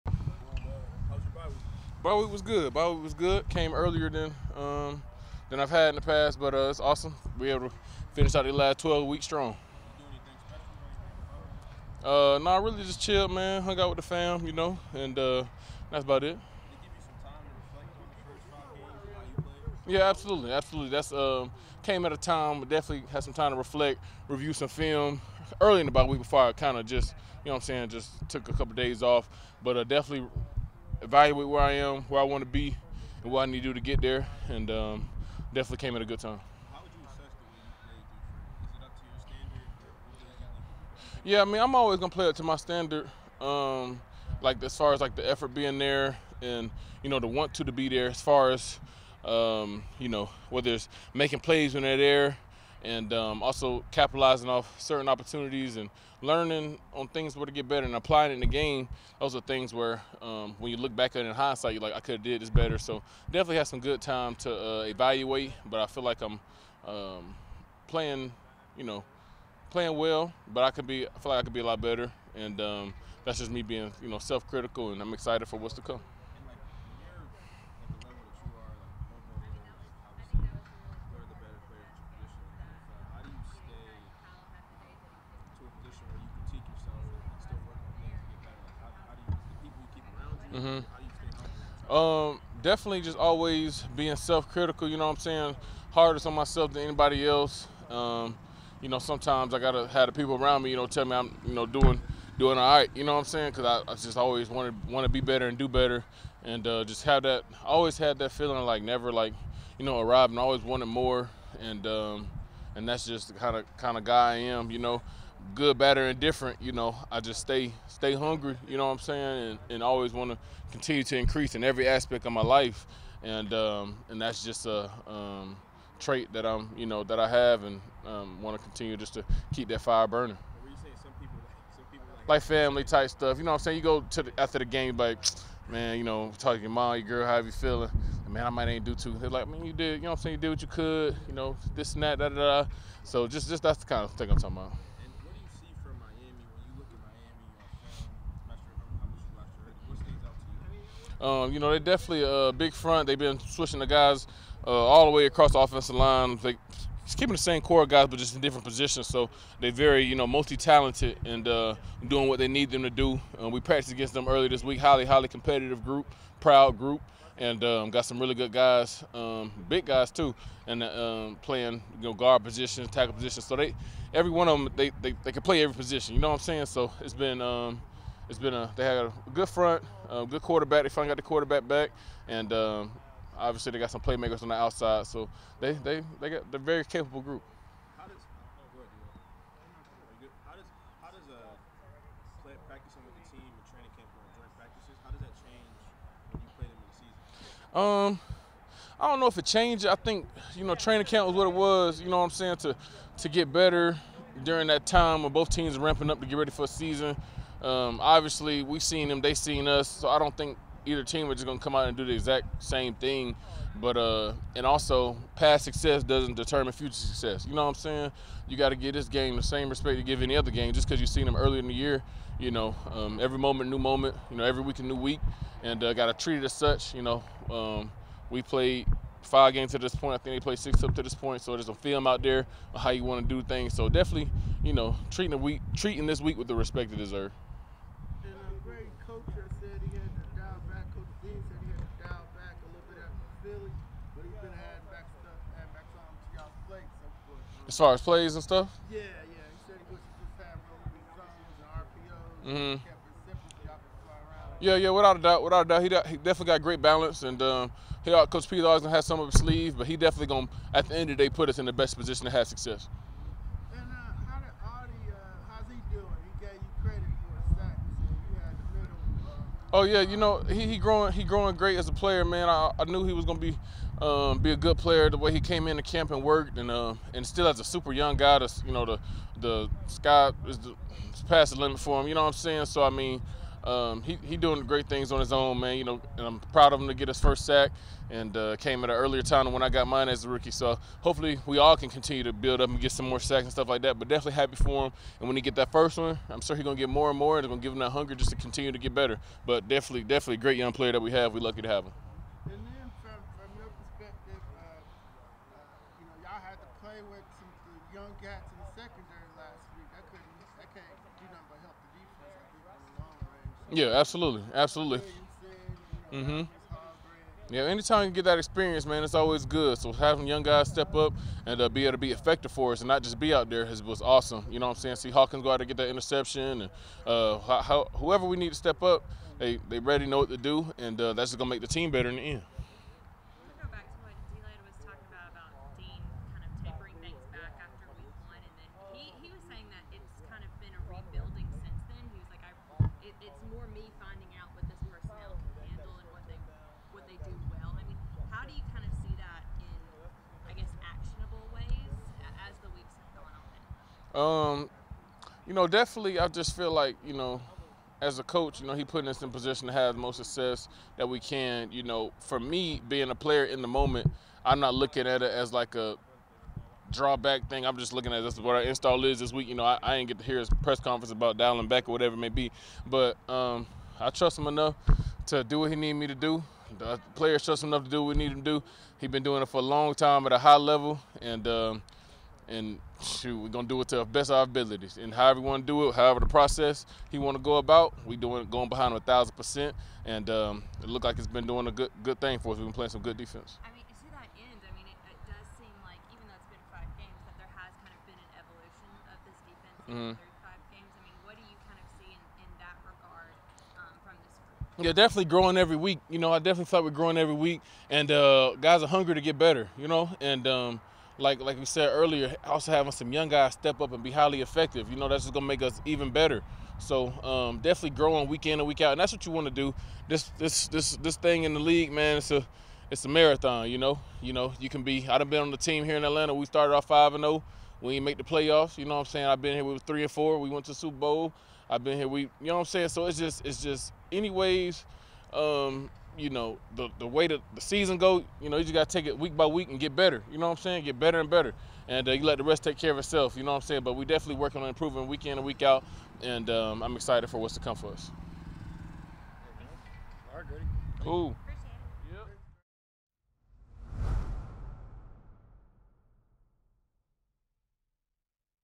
Grady Jarrett on how the defense is '1,000%' behind Dean Pees | Atlanta Falcons | NFL
Grady on what he did during his bye week, what he sees in the Dolphins' offensive line, and coach Pees putting the defense in the best position to have success.